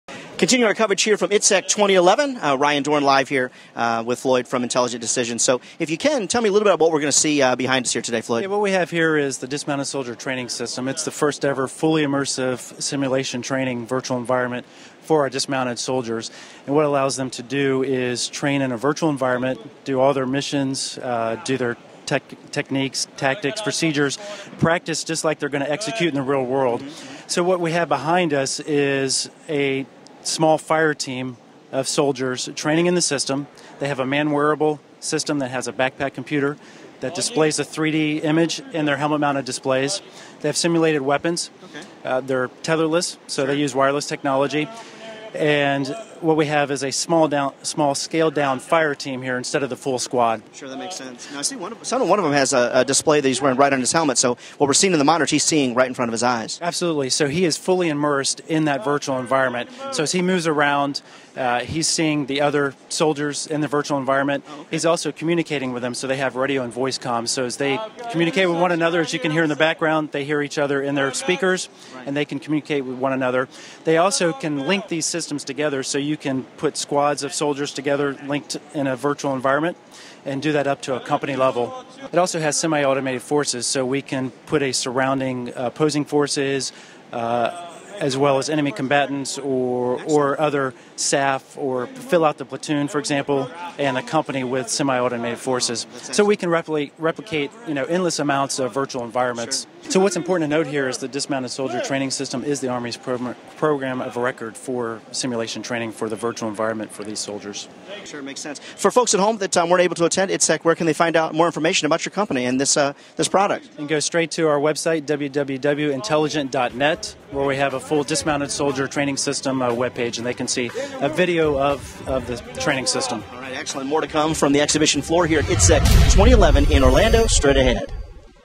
Intelligent Decisions, I/ITSEC 2011
At I/ITSEC 2011, Intelligent Decisions demonstrates their new Dismounted Soldier Training System's ability to provide a fully immersive environment with high fidelity graphics and technology that enables soldiers to virtually interact with their physical environment and use combat equipment.